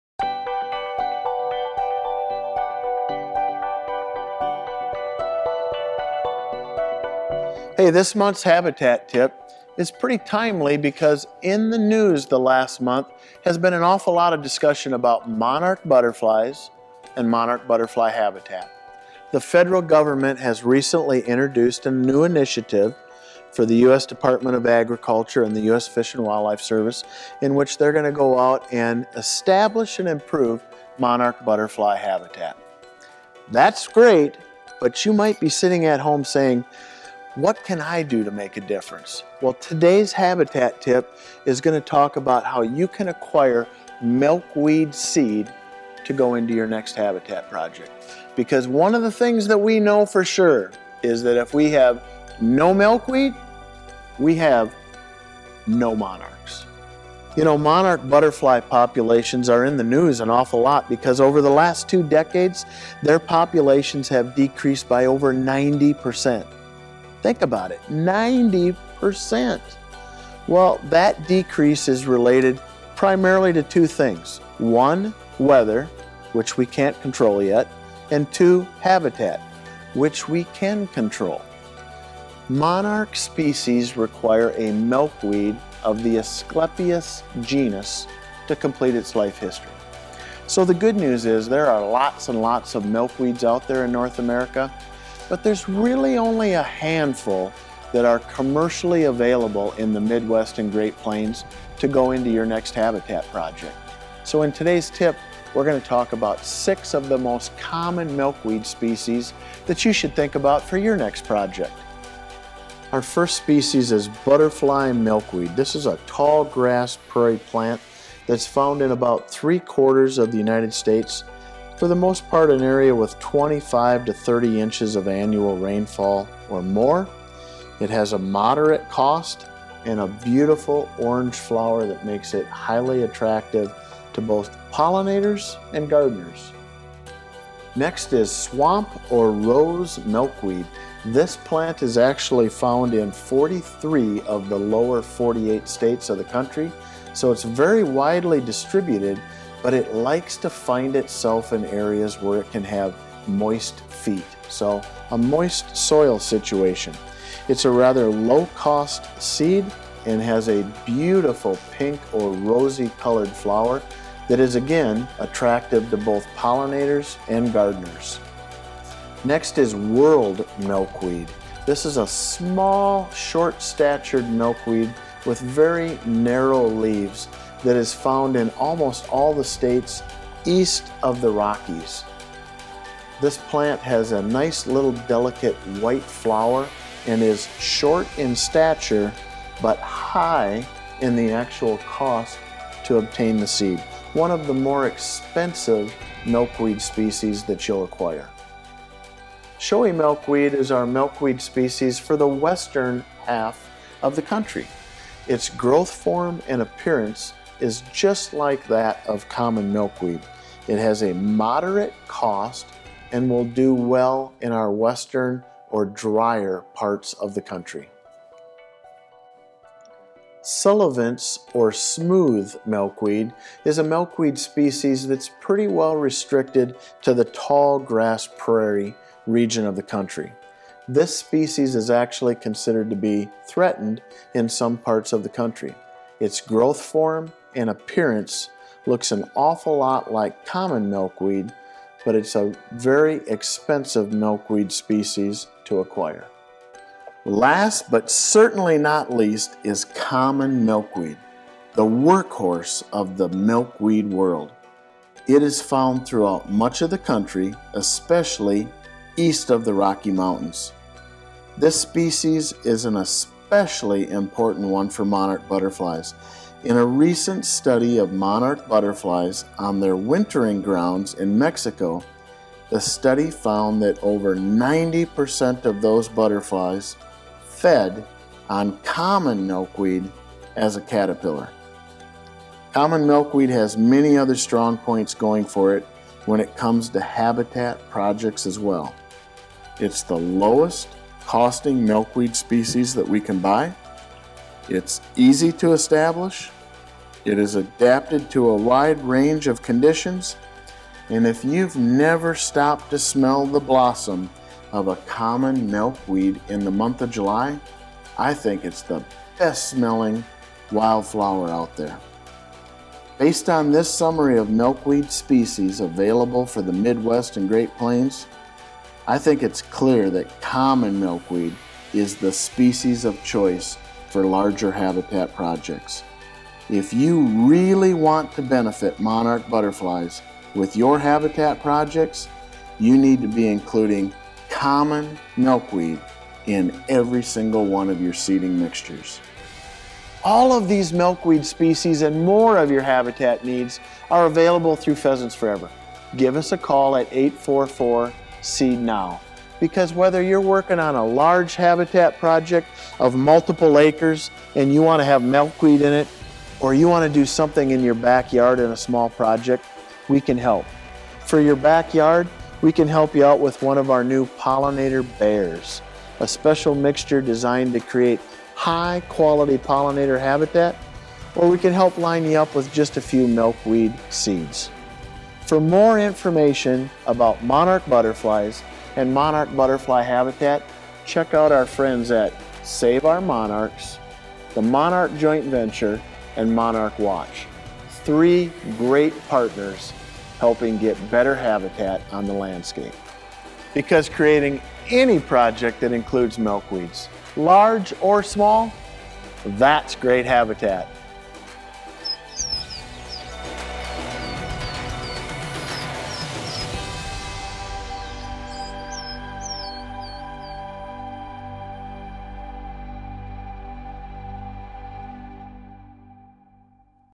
No Milkweed, No Monarchs
In this habitat tip, we discuss the importance of milkweed to the monarch butterfly population, as well as six different milkweed species that are commercially available to put in your next habitat project.